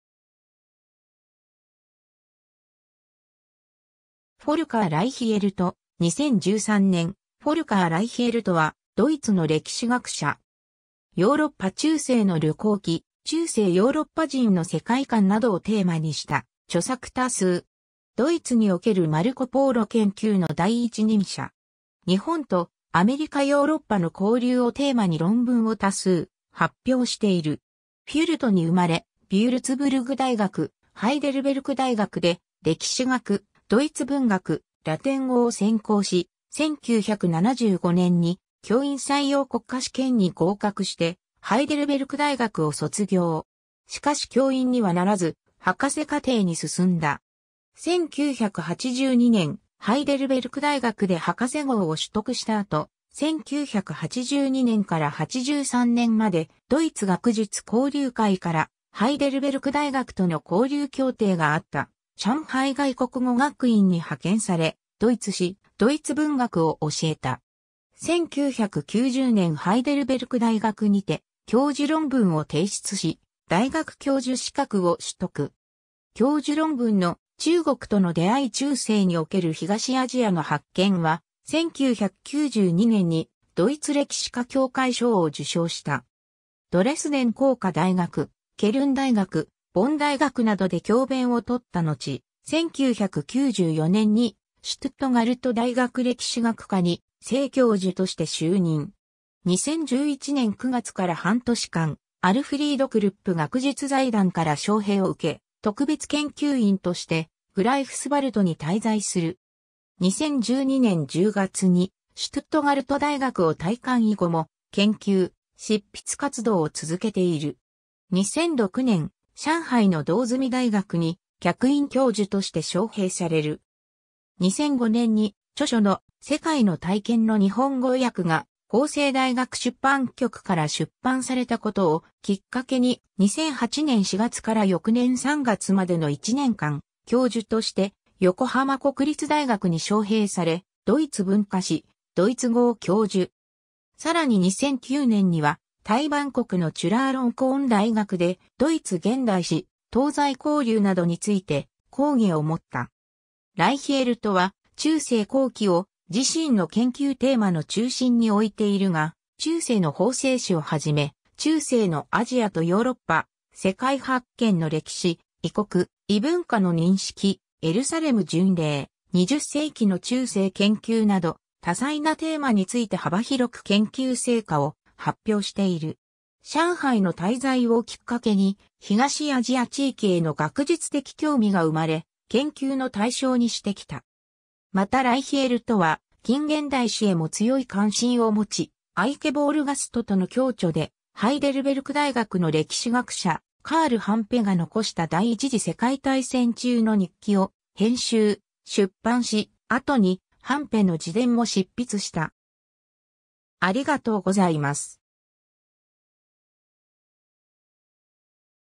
フォルカー・ライヒェルト
フォルカー・ライヒェルト、2013年 フォルカー・ライヒェルト（Folker Reichert, 1949年6月4日 - ）は、ドイツの歴史学者。
 ヨーロッパ中世の旅行記・中世ヨーロッパ人の世界観などをテーマにした著作多数。
ドイツにおけるマルコ・ポーロ研究の第一人者。
日本とアメリカ・ヨーロッパの交流をテーマに論文を多数発表している。
 フュルトに生まれ、ヴュルツブルク大学・ハイデルベルク大学で歴史学・ドイツ文学・ラテン語を専攻し、1975年に教員採用国家試験に合格してハイデルベルク大学を卒業。
しかし教員にはならず、博士課程に進んだ。
 1982年、ハイデルベルク大学で博士号を取得した後、1982年から83年までドイツ学術交流会 (DAAD) から、ハイデルベルク大学との交流協定があった上海外国語学院（現上海外国語大学）に派遣され、ドイツ史・ドイツ文学を教えた。
 1990年ハイデルベルク大学にて教授論文を提出し、大学教授資格を取得。
教授論文の「中国との出会い－中世における東アジアの発見」は、1992年にドイツ歴史家協会賞を受賞した。
ドレスデン工科大学・ケルン大学・ボン大学などで教鞭を執ったのち、1994年にシュトゥットガルト大学歴史学科に正教授として就任。
2011年9月から半年間、アルフリード・クルップ学術財団から招聘を受け、特別研究員 (Fellow) としてグライフスヴァルトに滞在する。
2012年10月にシュトゥットガルト大学を退官以後も、研究・執筆活動を続けている。
 2006年、上海の同済大学に客員教授として招聘される。
2005年に著書の『世界の体験』（原題 Erfahrung der Welt）の日本語訳が法政大学出版局から出版されたことをきっかけに、2008年4月から翌年3月までの一年間、教授として横浜国立大学に招聘され、ドイツ文化史・ドイツ語を教授。
さらに2009年にはタイ・バンコクのチュラーロンコーン大学でドイツ現代史・東西交流などについて講義を持った。
 ライヒェルトは中世後期を自身の研究テーマの中心に置いているが、中世の法制史を始め、中世のアジアとヨーロッパ、世界発見の歴史、異国・異文化の認識、エルサレム巡礼、20世紀の中世研究など、多彩なテーマについて幅広く研究成果を発表している。
上海の滞在をきっかけに、東アジア地域への学術的興味が生まれ、研究の対象にしてきた。
 またライヒェルトは近・現代史へも強い関心を持ち、アイケ・ヴォールガストとの共著で、ハイデルベルク大学の歴史学者カール・ハンペが遺した第一次世界大戦中の日記を編集・出版し、後にハンペの自伝も執筆した。
 （年代順）